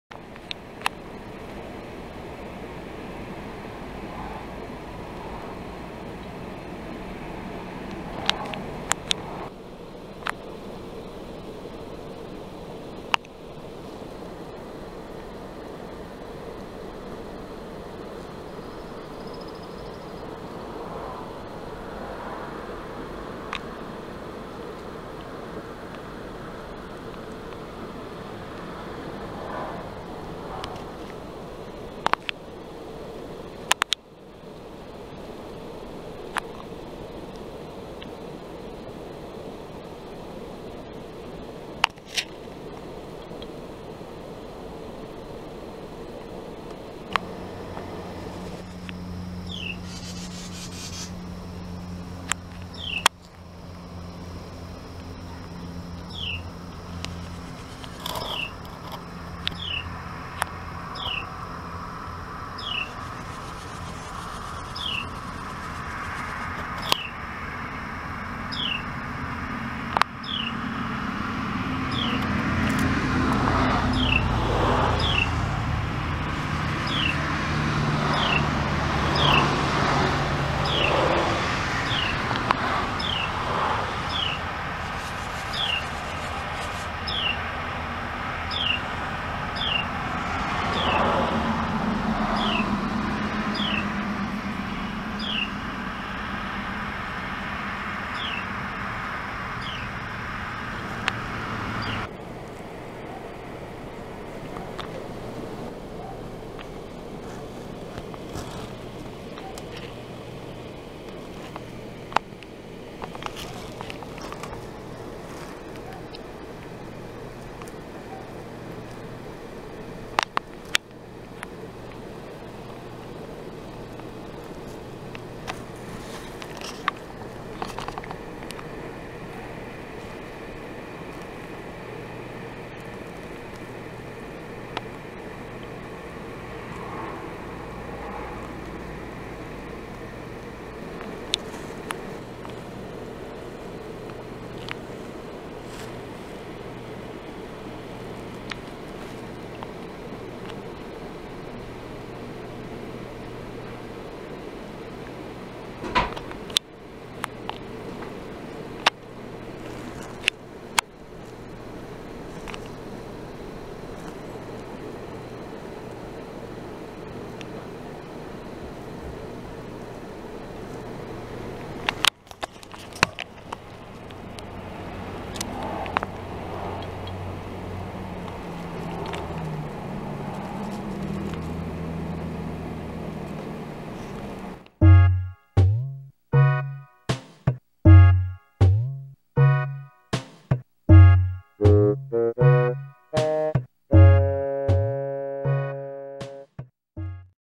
Noisy Mama Carolina Wren Feeding Her Babies
Plus a hummingbird at the end.
This is right outside my bedroom window. Loudest family ever